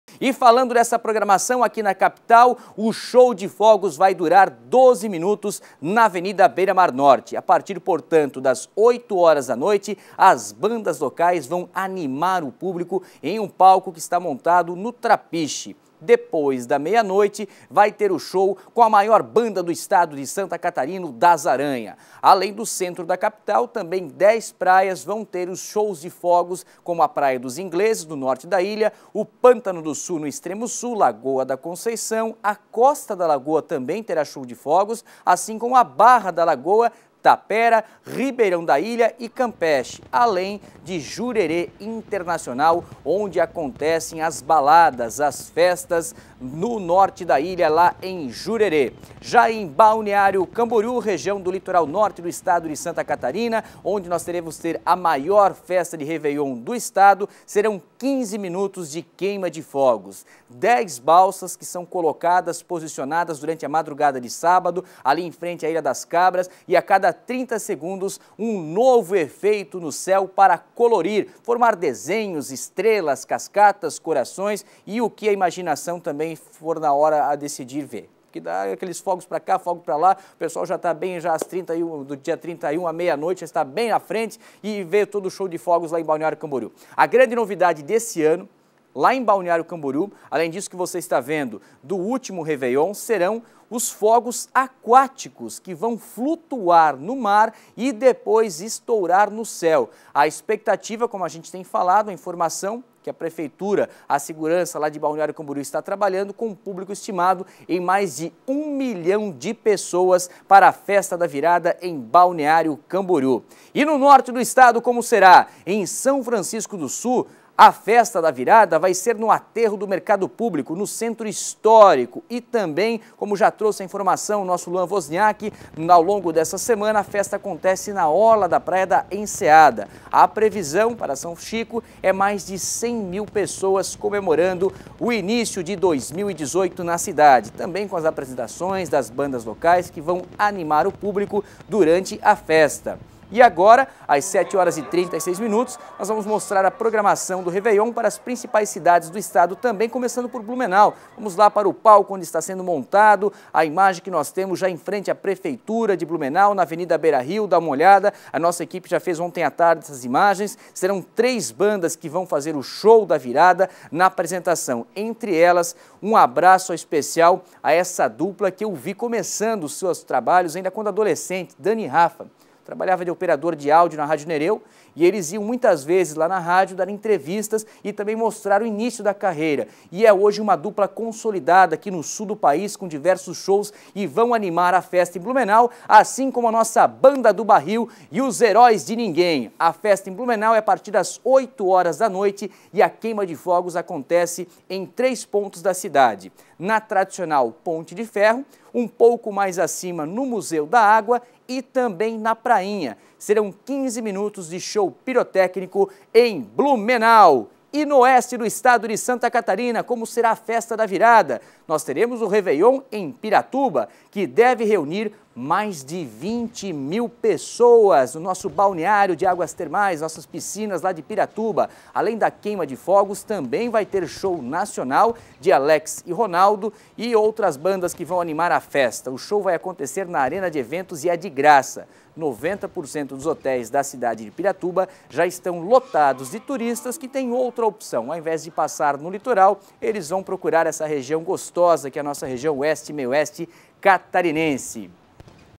Reveillon em Santa Catarina: confira a programação dos shows de fogos
Confira as novidades nos shows de fogos em Florianópolis, Balneário Camboriú, São Chico, Blumenau e Piratuba.